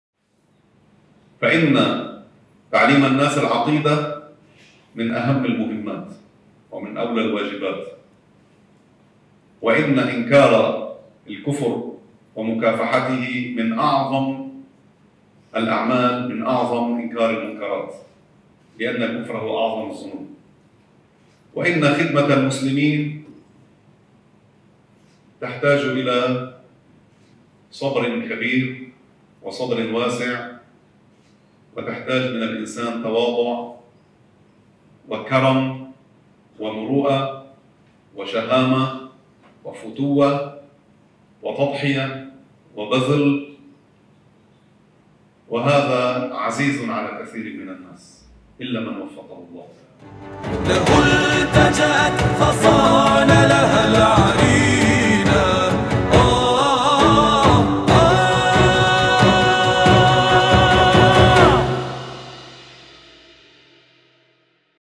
خدمة المسلمين
مقتطفات من كلام رئيس جمعية المشاريع الخيرية الإسلامية الدكتور حسام الدين قراقرة حفظه الله